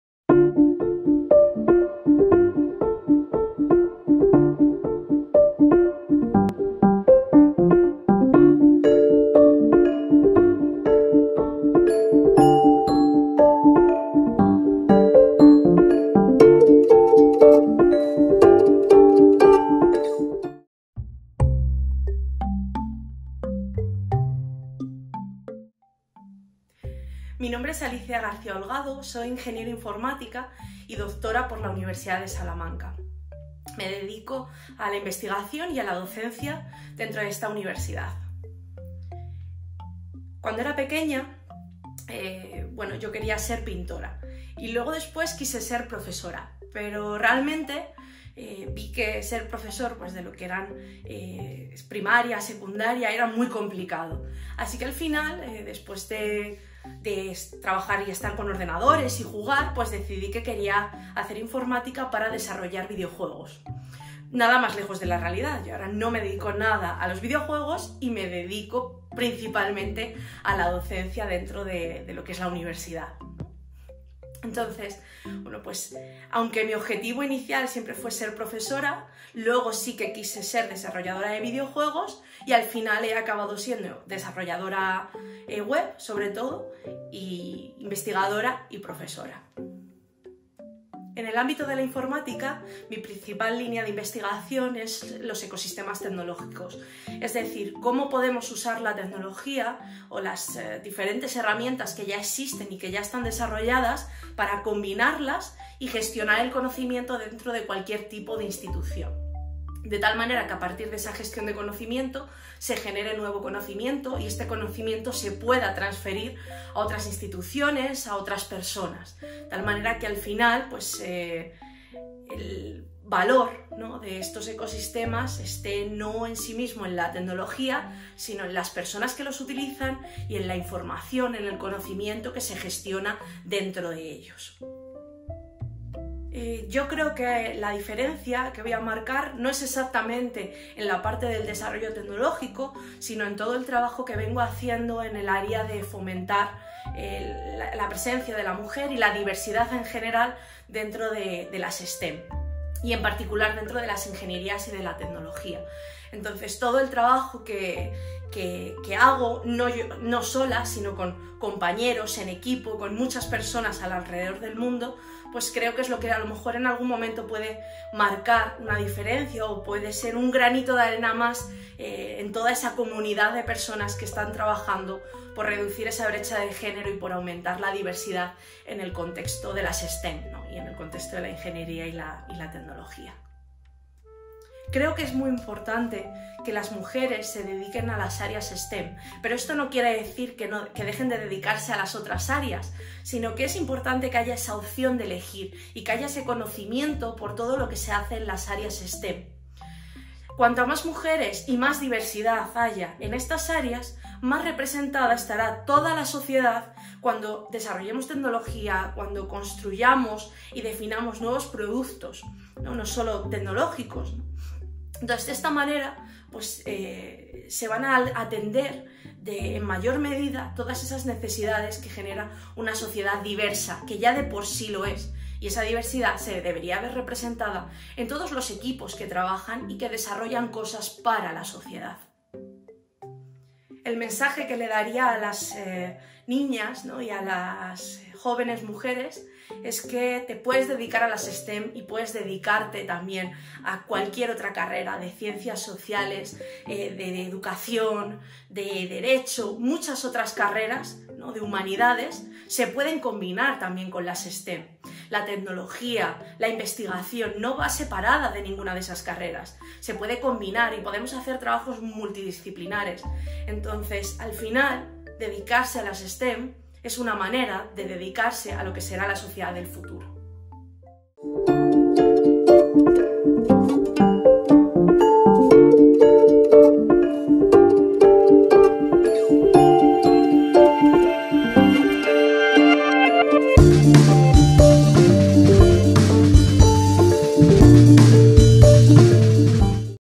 Mujeres W-STEM: Alicia García-Holgado
Alicia García-Holgado es profesora asociada en el Departamento de Informática y Automática e investigadora en el Grupo GRIAL de la Universidad de Salamanca (España).

Contenido producido por la Universidad de Salamanca como parte del proyecto W-STEM.
Subtítulos en Español revisados por Ana Ladrón de Cegama Casado y Sonia Benavent Galbis.